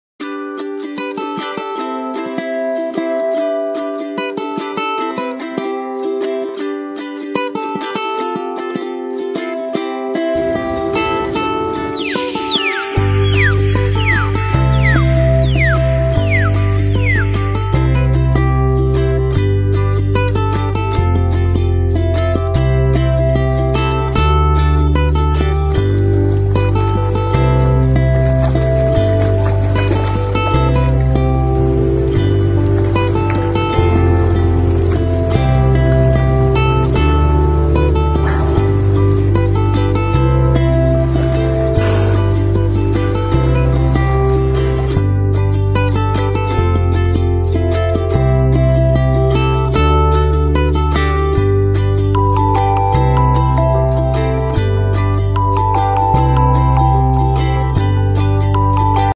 Ordos Zoo
Two white whales arrived in Ordos Zoo of north China's Inner Mongolia Autonomous Region in April this year. One is 11 years old and the other is 8. They often flirt with their keepers for small fish, "clap", spin in circles, "wave" when swimming, and play pranks.